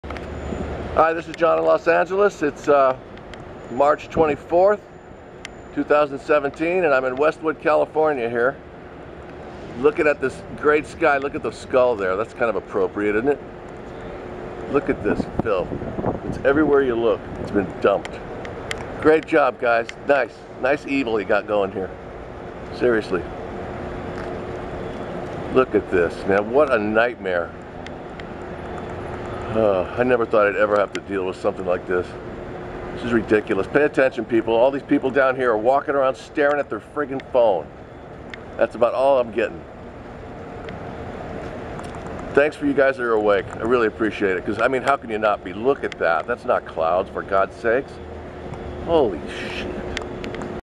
Sick Skies Over Westwood Ca. Mar. 24, 2017
Come on people! Please look at your skies. There are nothing you grew up with. Nothing you see here is natural clouds..at all.